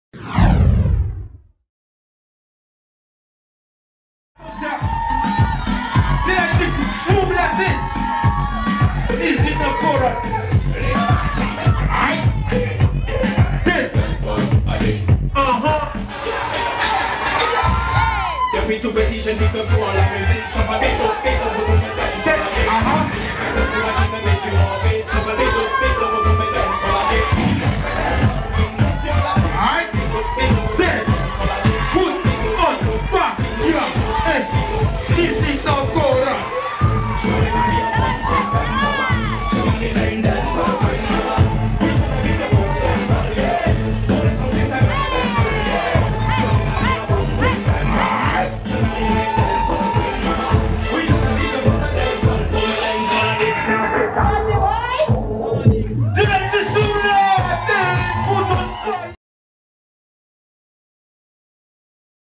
"DHQ & DHK CIS 2016" - DHK 2nd round - DENIS FOOTONFAYA
"DHQ & DHK CIS" is an annual international dancehall contest organized by BIG UP PRODUCTION. It includes DHQ, DHK contest and DANCEHALL ADDICT 1vs1 battle for PRO & BEGINNERS.